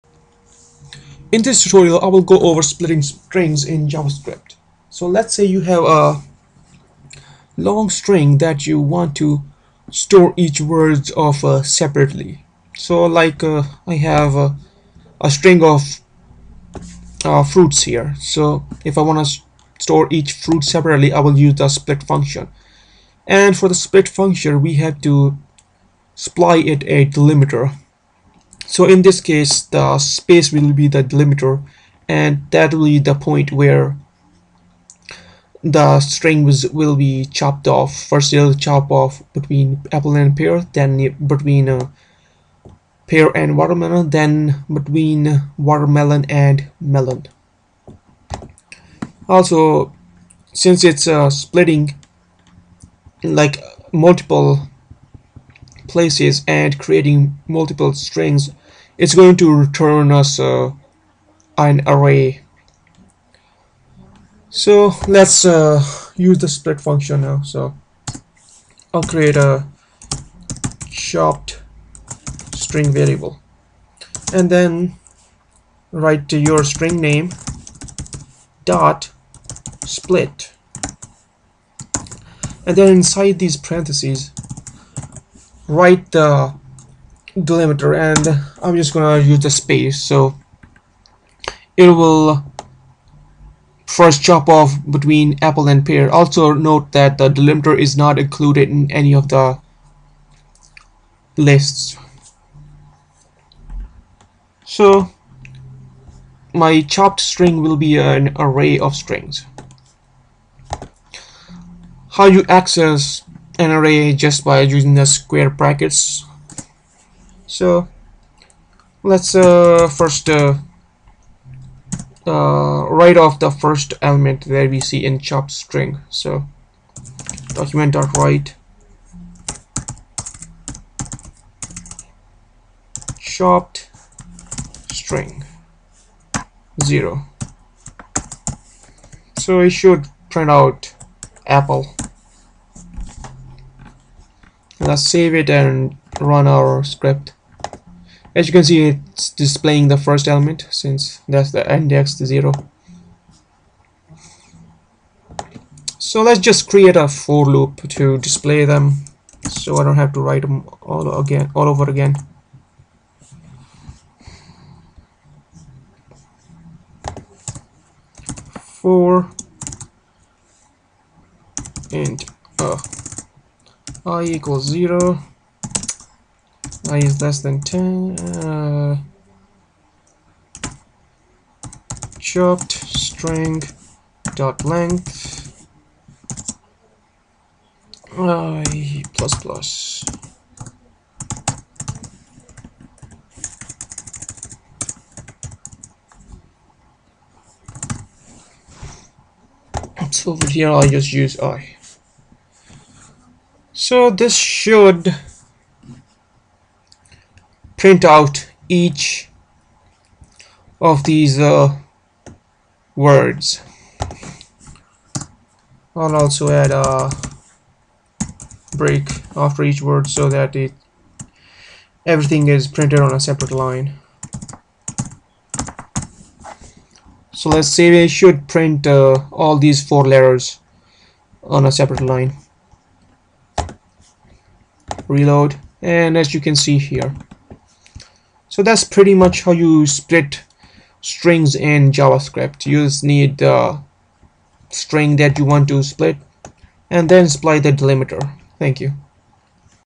JavaScript Tutorial 36 - Split Strings in Javascript
In this video tutorial I go over how to Split JavaScript strings. Sometimes you need to split strings to get part of the string for calculations or other purposes, so JavaScript and many other programming languages provide splitting functions.